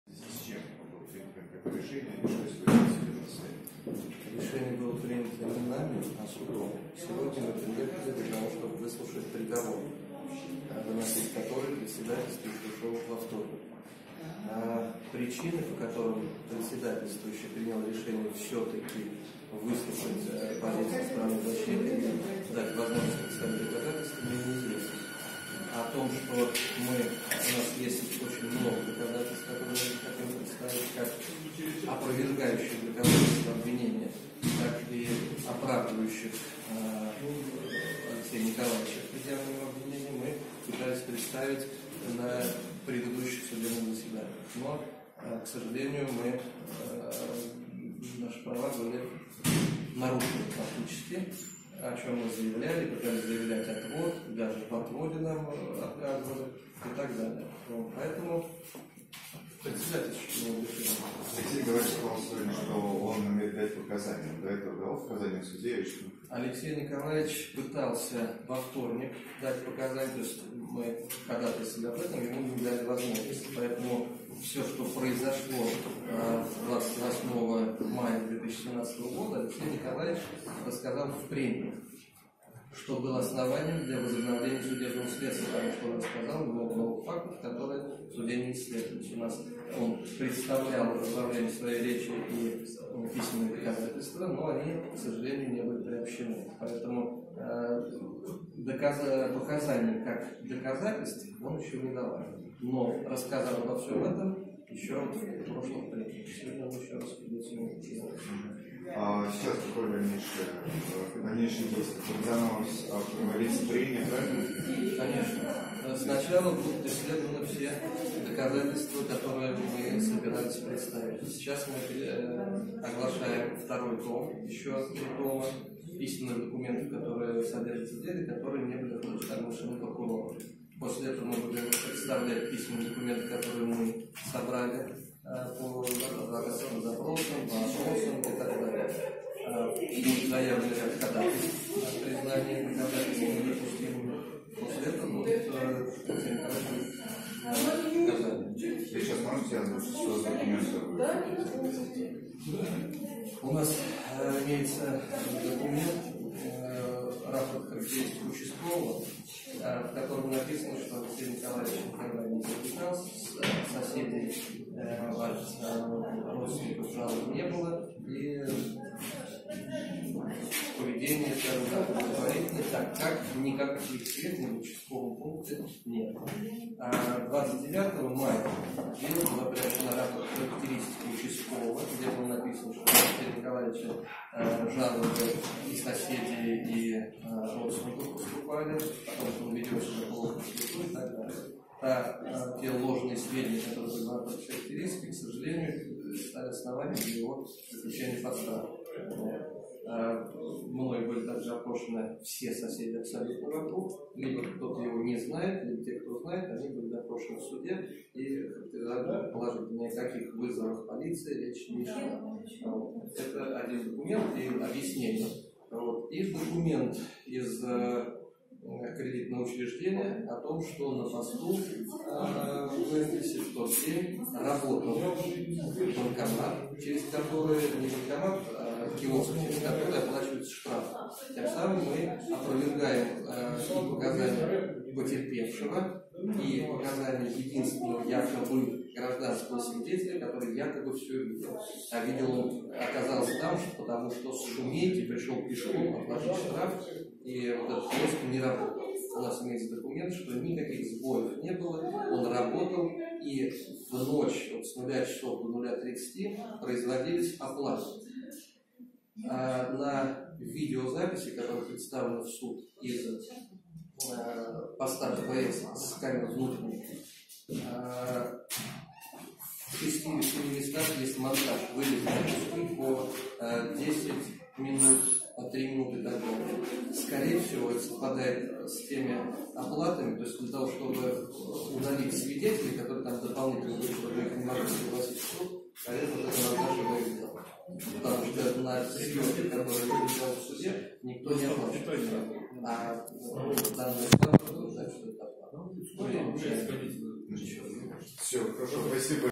Адвокат Алексея Ерусланова прокомментировал возобновление судебного следствия
Сегодня, 16 февраля, в Энгельсском районном суде планировалось оглашение приговор по делу предпринимателя Алексея Ерусланова, обвиняемого по части 2 статьи 318 УК РФ (Применение насилия к представителю власти). Судья Елена Кулишова в начале судебного заседания заявила, что процесс будет возвращен на этап следствия в связи с необходимостью изучить показания подсудимого.